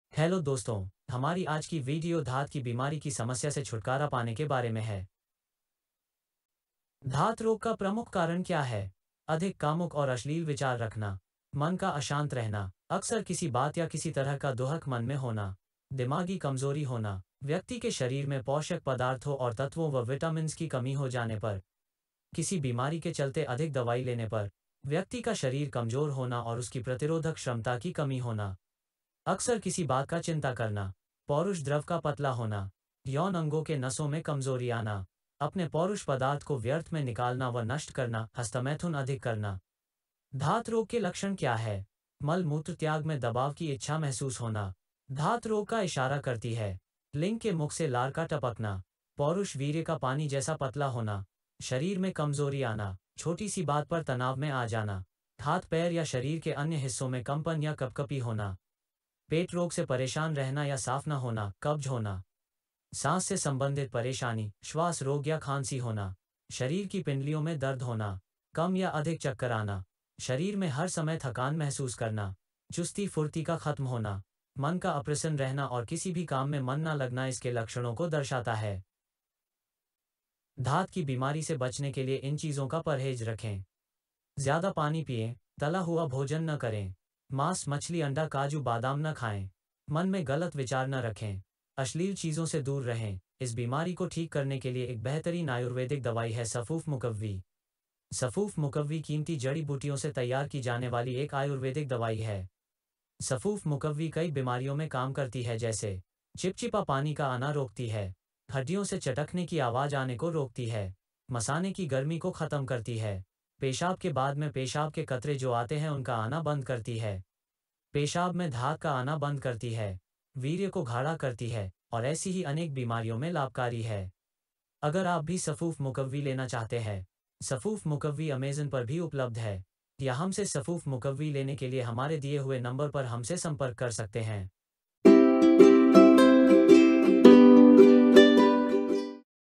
आयुर्वेदिक दवा सफूफ मुकव्वी से धात रोग का उपचार इलाज। अमेज़न से खरीदें
सफूफ मुकव्वी एक ऐसी आयुर्वेदिक दवा है जिसके इस्तेमाल से धत रोग चिपचिपापानी आना १० दिन में बंद हो जाता है।
मसानी की गर्मी को ख़तम करती है।  धात रोग को समाप्त करती है और टाइमिंग बढाती है।
ये दवा ऑनलाइन भी उपलब्ध है।  आप निचे दिए गए लिंक पे क्लिक करके आर्डर कर सकते हैं।  या  फिर स्क्रीन पे दिए नंबर पे कॉल करके आर्डर कर सकते है।